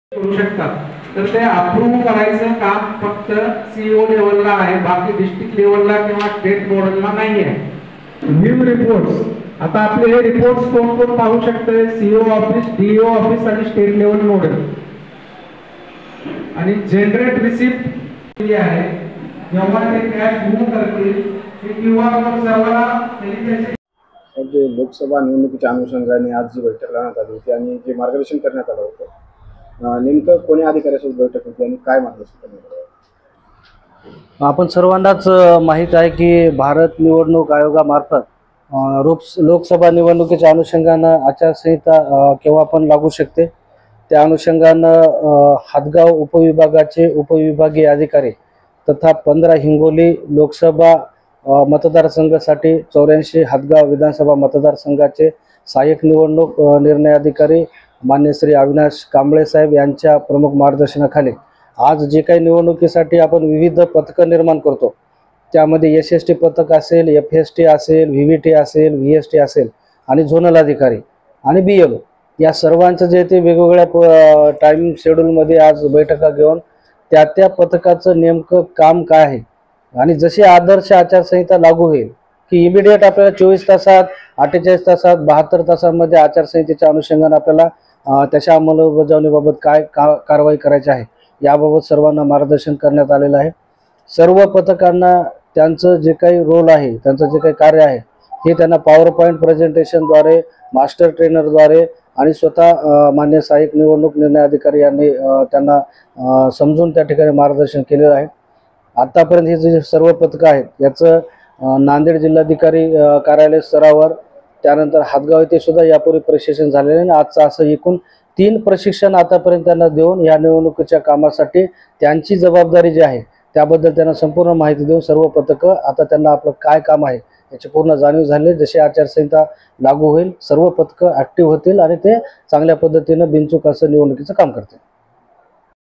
NANDED | लोकसभा सर्वत्रिक निवडणुकीसंदर्भात हदगाव तहसील कार्यालयात मार्गदर्शन बैठकीचे आयोजन
भारत निवडणूक आयोगामार्फत लोकसभा निवडणुकीच्या अनुषंगाने आचारसंहिता केव्हा पण लागू शकते त्या अनुषंगाने हादगाव उपविभागाचे उपविभागीय अधिकारी तथा 15 हिंगोली लोकसभा मतदारसंघासाठी 84 हादगाव विधानसभा मतदारसंघात सहाय्यक निवडणूक निर्णय अधिकारी अविनाश कांबळे साहेब यांच्या प्रमुख मार्गदर्शनाखाली आज हदगाव तहसिल कार्यालयात बैठक पार पडली. काही निवडणुकीसाठी  विविध पथक निर्माण करतात त्यामध्ये यशस्वी पथक असेल व्ही.व्ही.टी असेल आणि झोनल अधिकारी सर्वांचे येथे वेगवेगळ्या टायमिंग शेड्युलमध्ये आज बैठका घेऊन त्या त्या पथकाचे नेमके काम काय आहे आणि जसे आदर्श आचारसंहिता लागू झाल्यानंतर त्या अनुषंगानेत्याच्या अंमलबजावणीबाबत काय कारवाई करायचे आहे याबाबत सर्वांना मार्गदर्शन करण्यात आले असल्याची माहिती हदगावचे तहसिलदार विनोद गुंडमवार यांनी दिली आहे.

BYTE- विनोद गुंडमवाड, तहसीलदार हदगाव